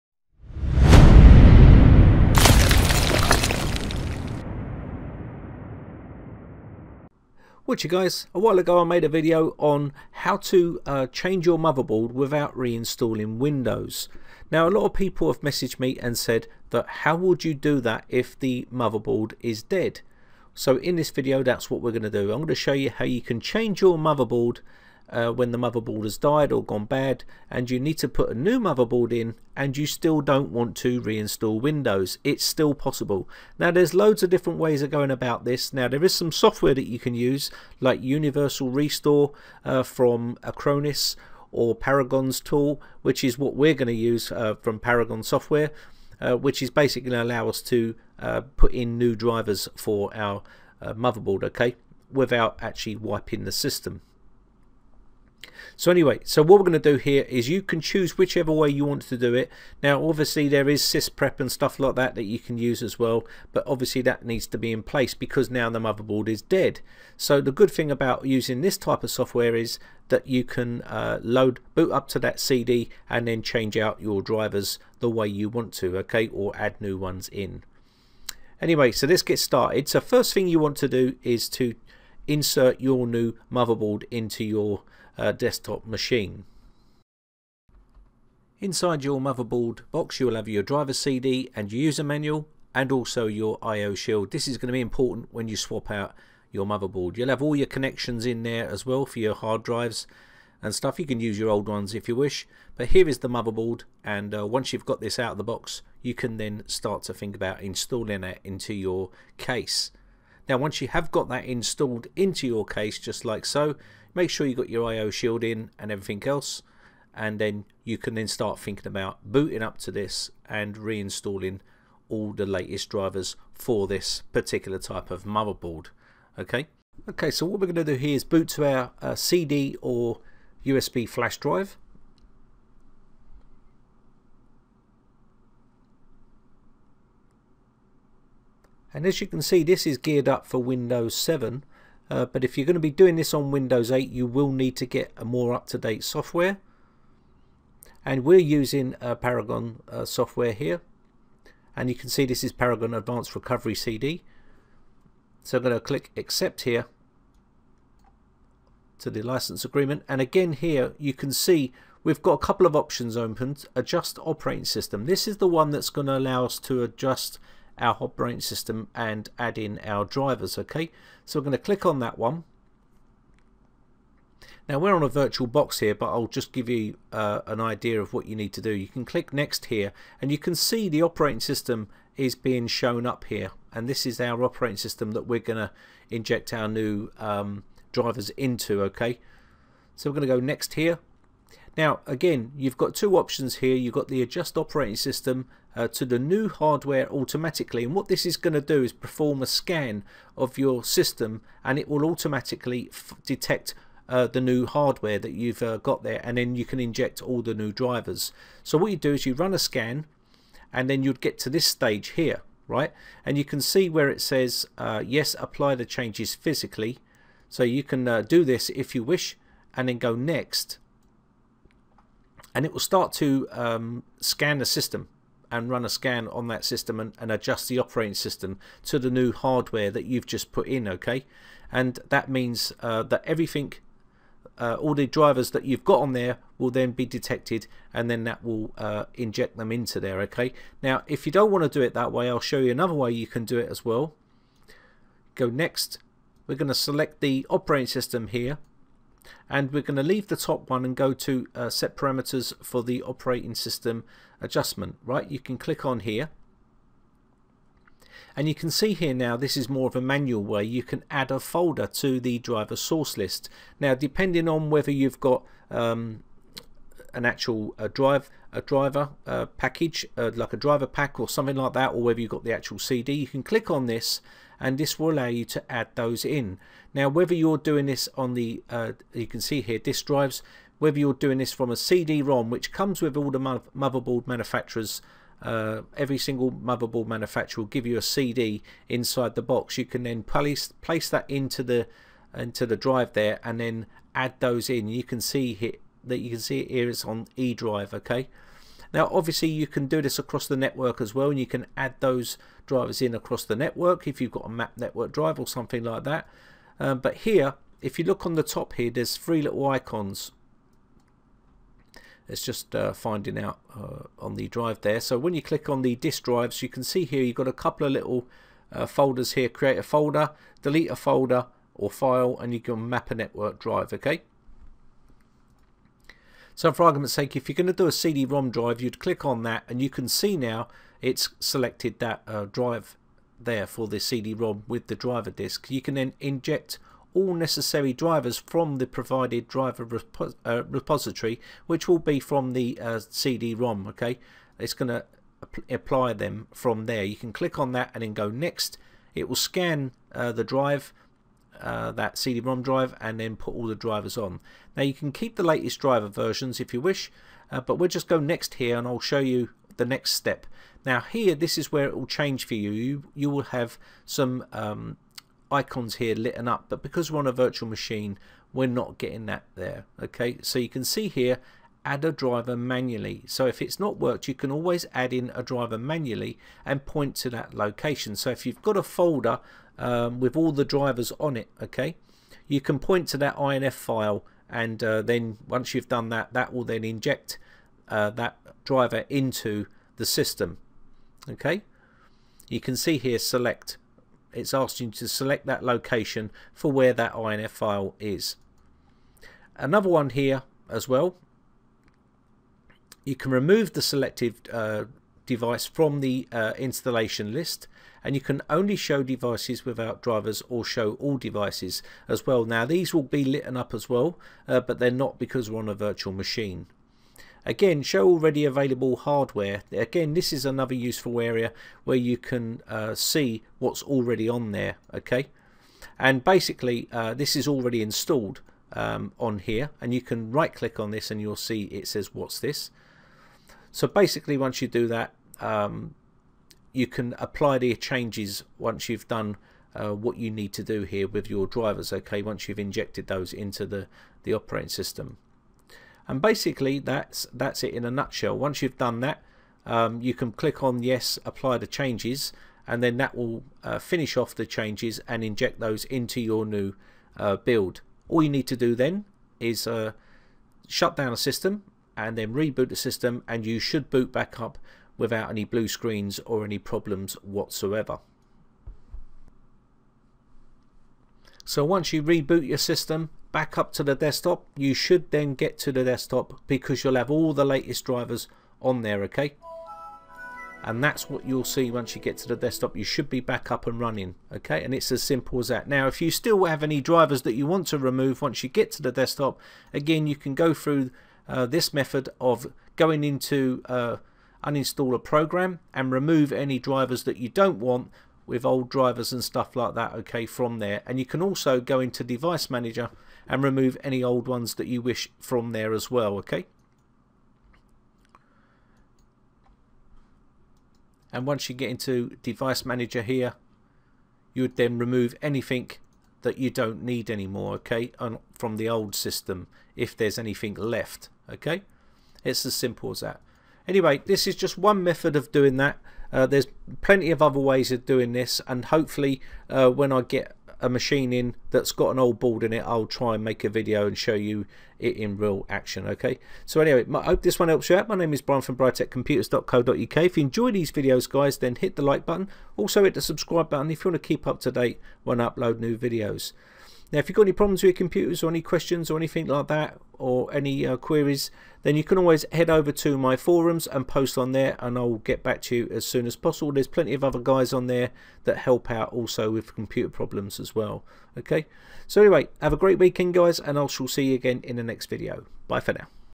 Replace Dead Motherboard Without Reinstalling Windows
In this video I will show you one way to change your motherboard if it has failed / bad and needs replacing, we need software to do this so we can delete old drivers and replace with new driver.

you can use any type of software you want to do this job. Acronis universal restore, Paragon Adaptive Restore or P2P Adjust OS Wizard. it does not matter what software you use. I still think its best to wipe the computer and reinstall with a fresh copy of windows.